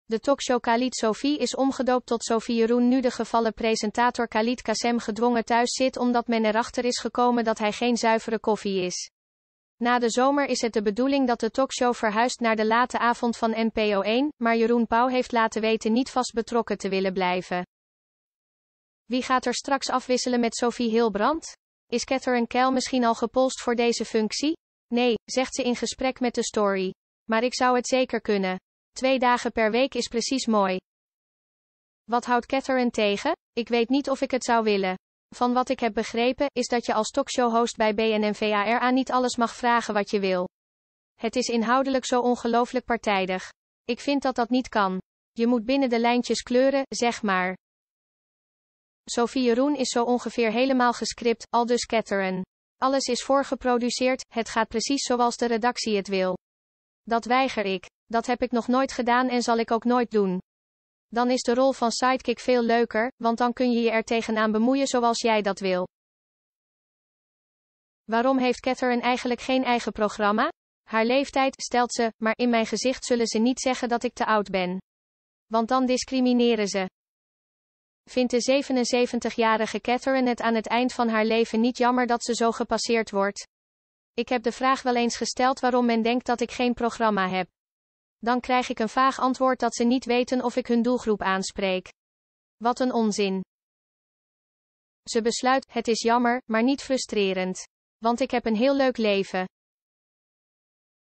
Catherine Keyl weigert presentatie Sophie & Jeroen: ‘Gemuilkorfd’
Catherine Keyl weigert de presentatie van de talkshow Sophie & Jeroen als ze gevraagd wordt om de permanente vervanger van Khalid Kasem te worden, zegt ze. “Het is zó partijdig.”